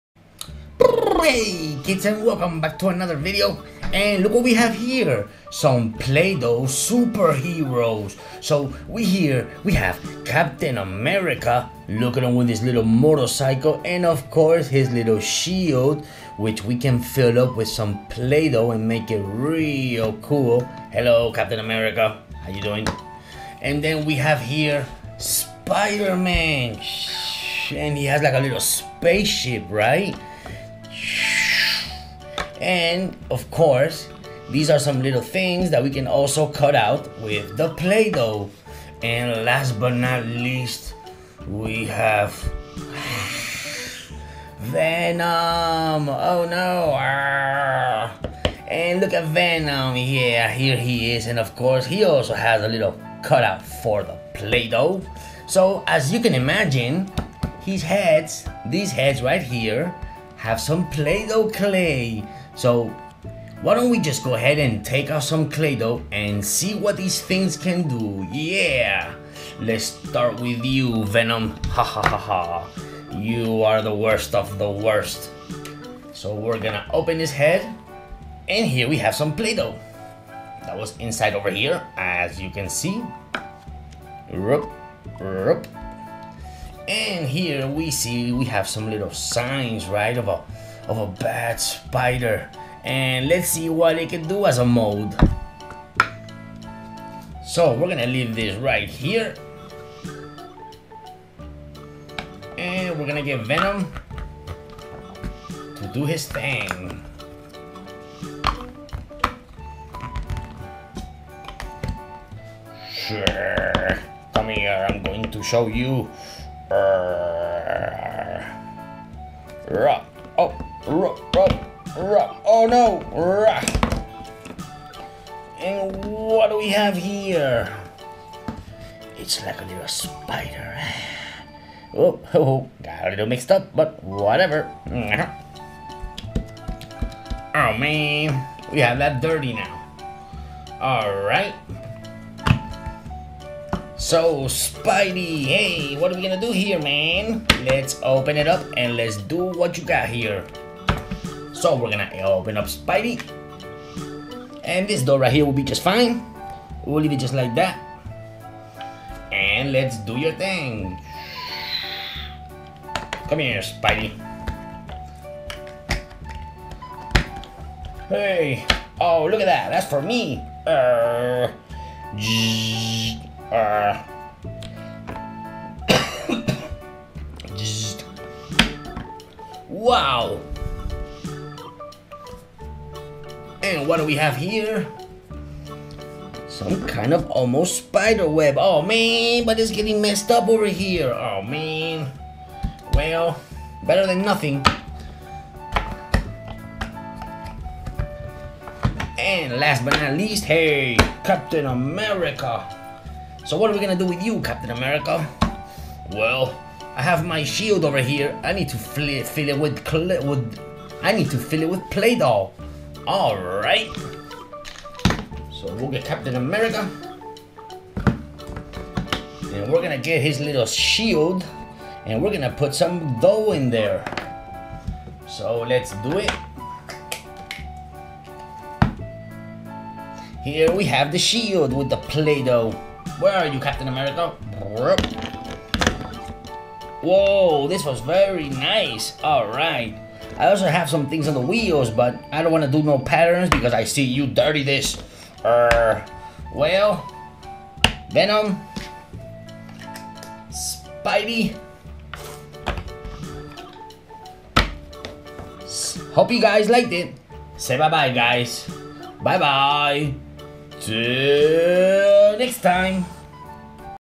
Spiderman Spaceship Venom Captain America Motorcycle Play-Doh Superheroes
Hi check out this Spiderman with his airplane,  Venom and Captain America motorcycle with Play-Doh Superheroes fun